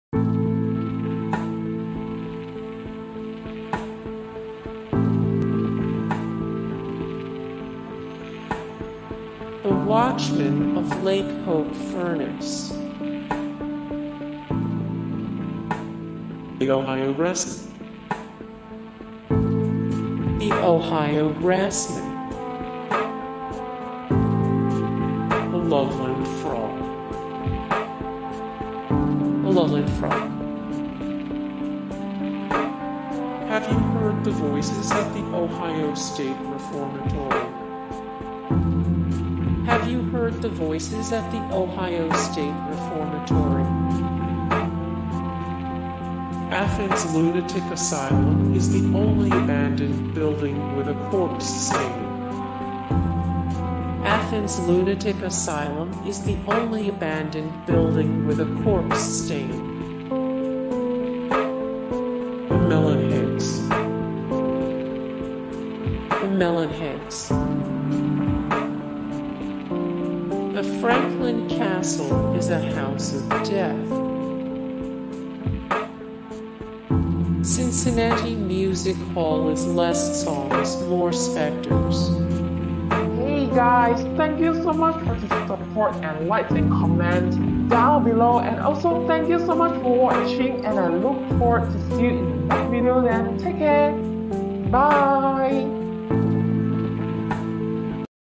12 Ghost Stories And Urban Legends That Prove Ohio Is The Creepiest State 💎
When you look up all the different ghost stories from Ohio, you realize the Birthplace of Aviation boasts enough creepy content to warrant more attention than a fewattack ads every election cycle. Ohio urban legends read like a greatest hits of the urban legends of our country. You looking forBigfoot? You got it.How about a bridge featuringthe screams of drowning children? Sure. What about an entire town thats full of every urban legend trope known to man? The Buckeye State's got you, boo.When it comes to getting spooky-ooky, Ohio delivers. Along with some genuinely frightening ghost stories, the Buckeye statehostsits share of quirky cryptidsyou wont find anywhere else.
Ghost stories from Ohio form the basis for a lot of the urban legends you hear in modern-day America. Whether youre listening to a story about an old night watchmantasked with guarding a run-down ore refineryor a haunted reformatory, these are the kinds of stories makingup the backbone of Americas hauntings. Its possiblethe ghost stories of Ohio influenced the rest of the country'stales, or maybe the people who live in the heart of italso tellall the scariest stories. Either way, there exists a wonderful cross section of hauntings, urban legends, and straight-up weird stuffin Ohio.
Photo: Metaweb/CC-BY
0:00 - Intro
0:00:08 - The Watchman Of Lake Hope Furnace
0:00:16 - The Ohio Grassman
0:00:26 - The Loveland Frog
0:00:35 - Have You Heard The Voices At The Ohio State Reformatory?
0:00:49 - Athens Lunatic Asylum Is The Only Abandoned Building With A Corpse Stain
0:01:06 - The Melonheads
0:01:15 - The Franklin Castle Is A House Of Death
0:01:23 - Cincinnati Music Hall Is Less Songs, More Specters